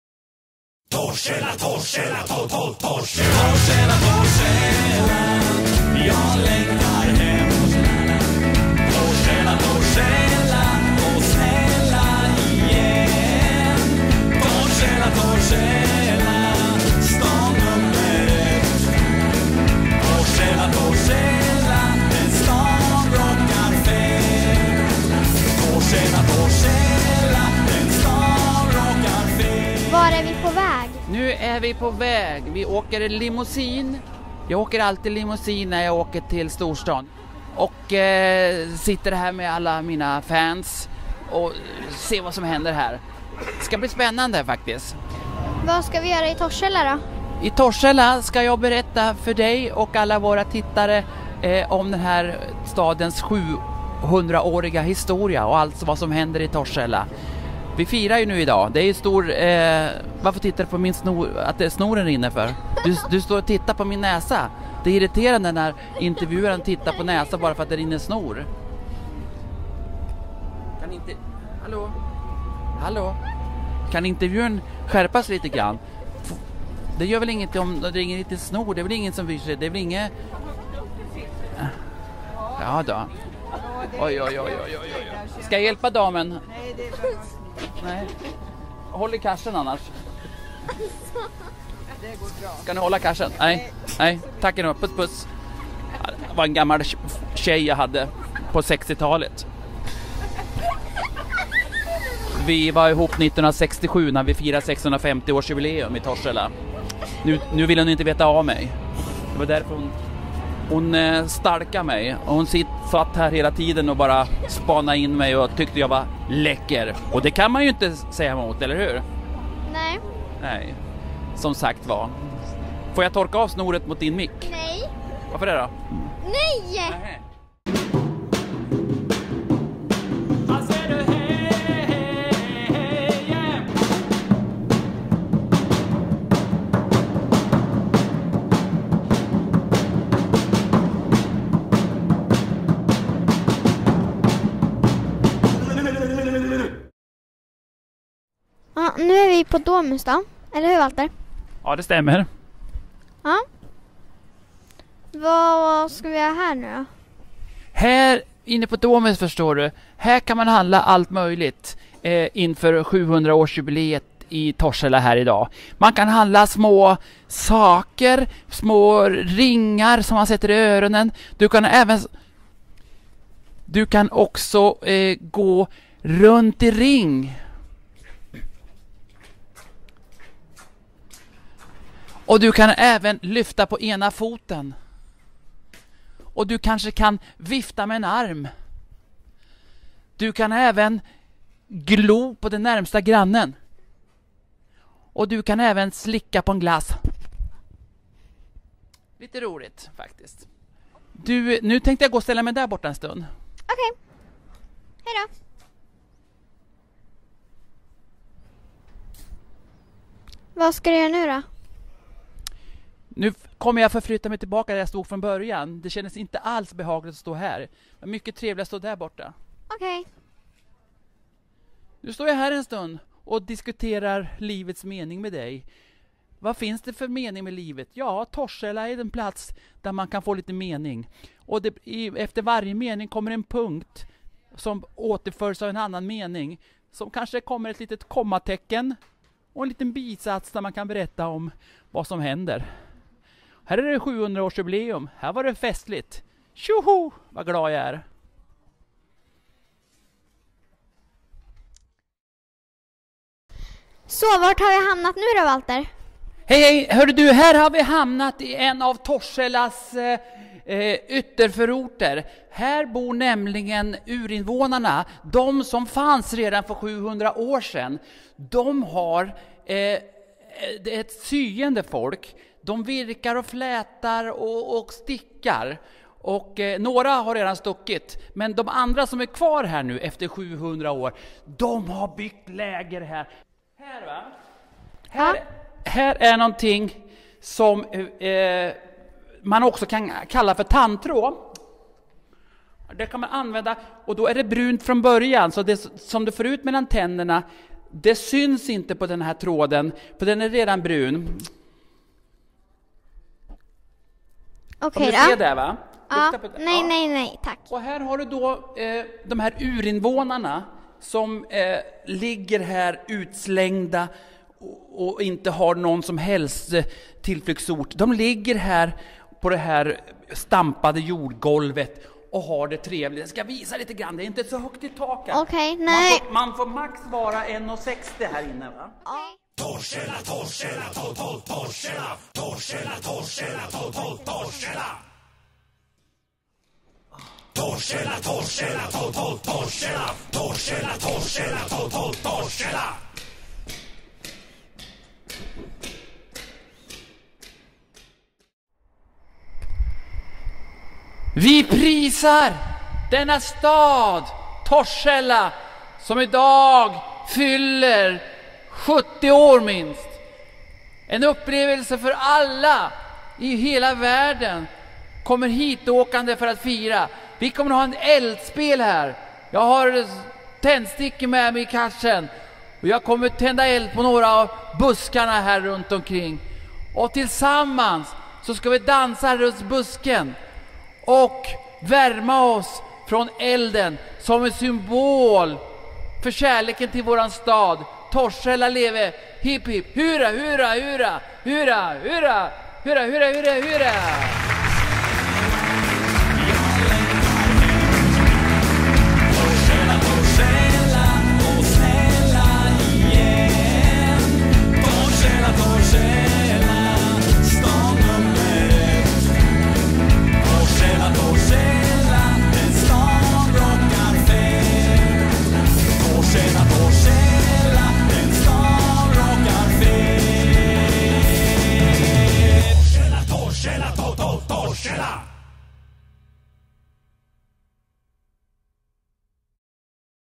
Torshällas 70-årsjubileum... eller är det 700 år?
En film av och med Walter Kurtsson och Elsa Torsén gjord som en hyllning till den gamla staden TORSHÄLLA. Staden som skapade Walter.
© Zero Zero Musik & Komik AB 2017. Mer Walter på Sök "Walter Kurtsson" på Spotify och Apple Music.